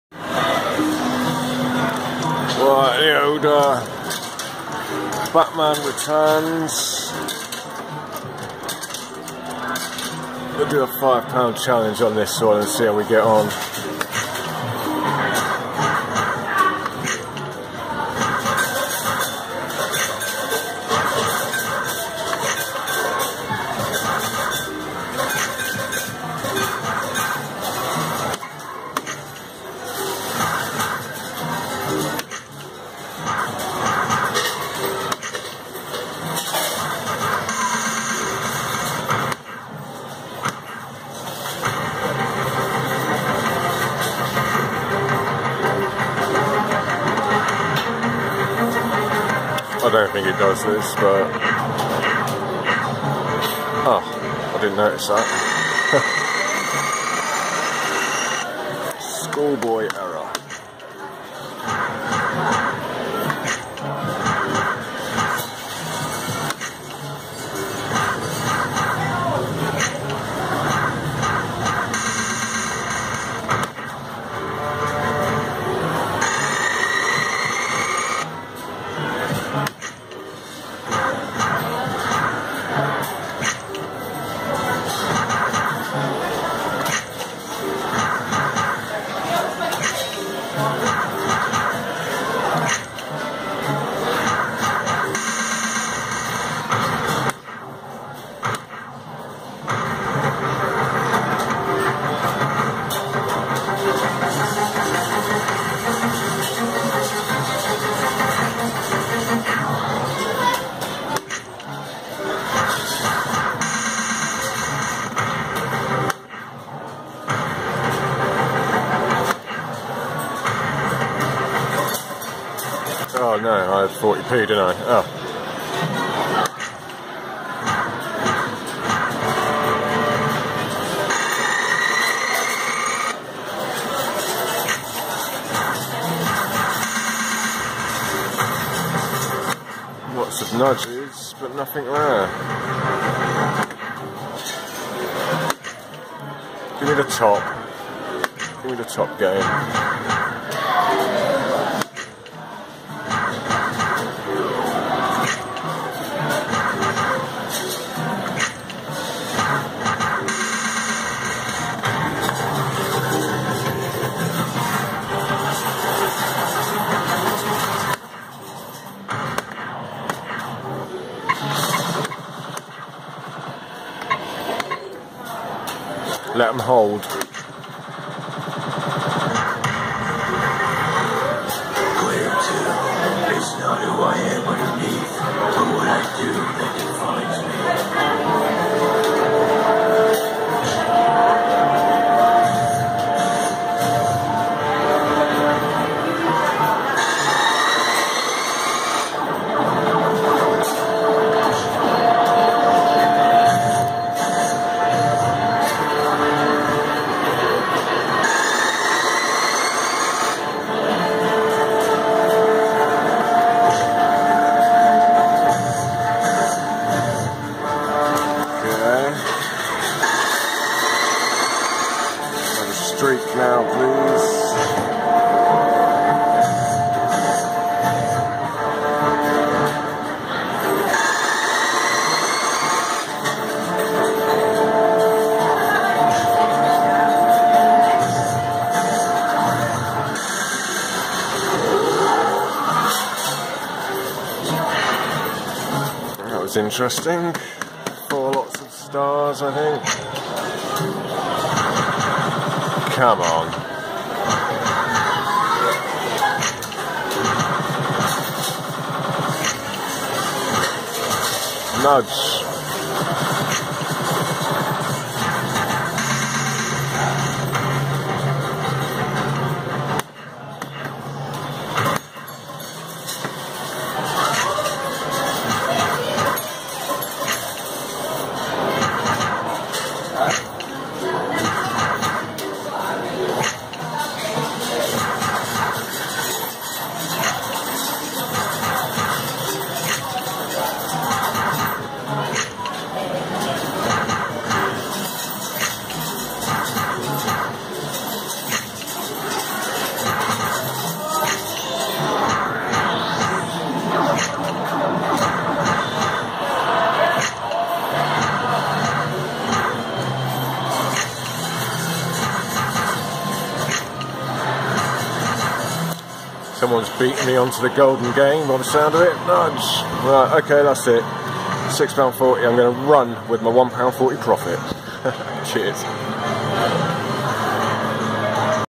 BATMAN BEGINS fruit machine.£5 challenge
Love the sound effects on this machine!!
Is there a profit from this £5 challenge??? Let's find out.

LESUIRE 2000 Torbay road, paignton.